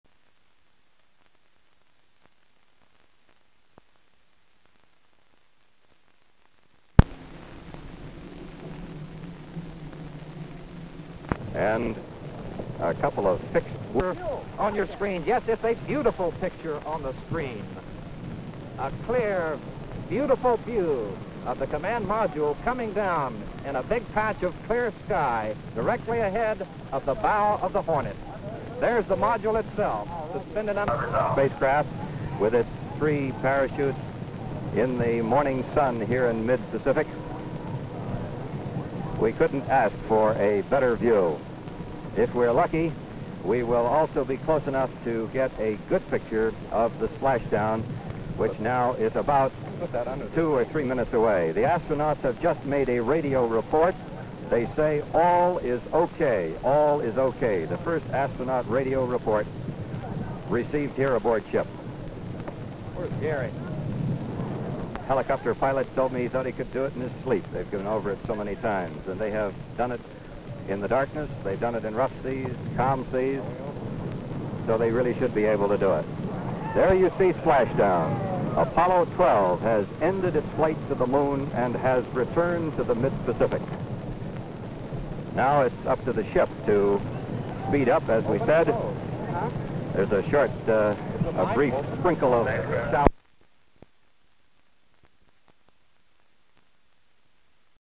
APOLLO 12 SPLASHDOWN - SOUND - COLOUR
Mid Pacific. Hornet aircraft carrier - HAS large number of men on deck. LS of helicopter in the sky. Command module is seen descending. Camera follows the capsule as it descends suspended by three parachutes. LS of capsule descending. Splashdown. LS of parachutes floating down to the sea.